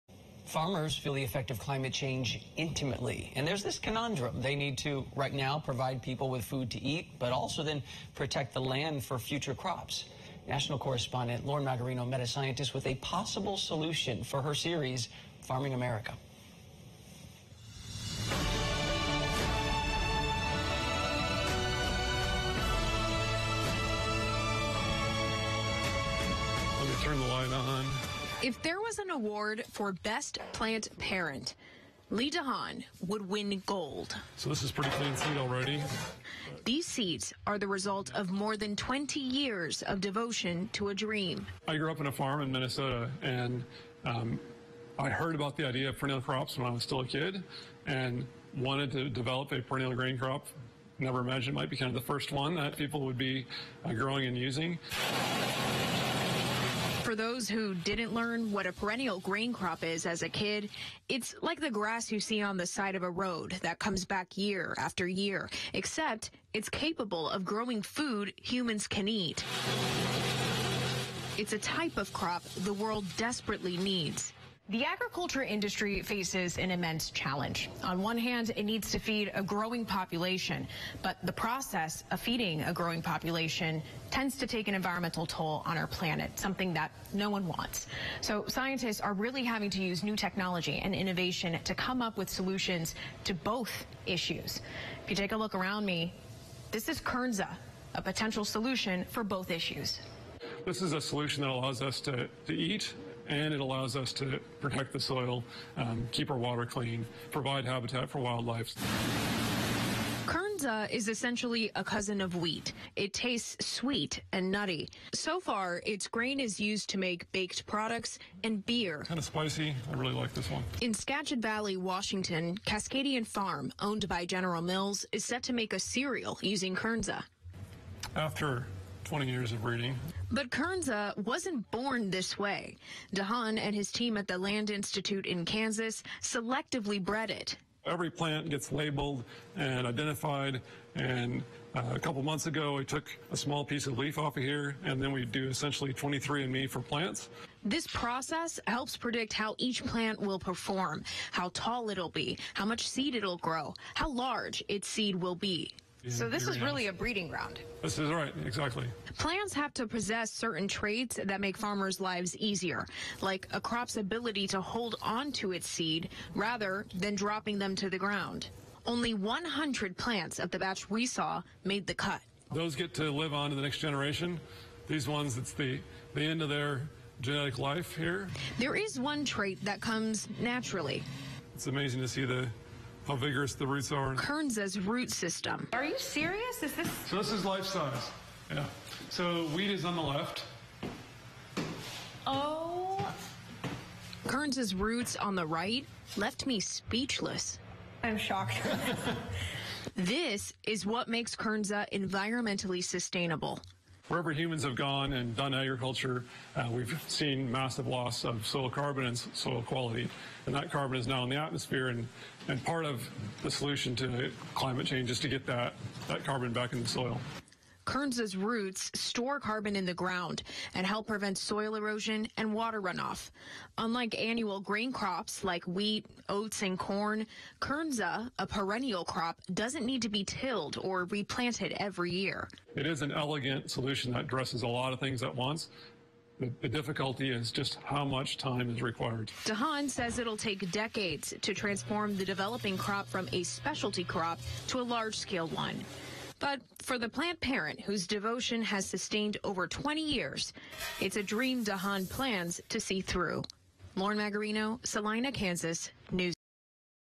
This Earth Friendly Crop May Change Agriculture For The Better
Kernza is a perennial crop that's essentially a cousin of wheat, but its roots store carbon in the ground to help prevent soil erosion and runoff.

Lauren Magarino takes a trip to visit Scientist Lee Dehaan in Salina, Kansas to learn about how DeHaan and his team at The Land Institute in Kansas selectively bred the perennial grain crop.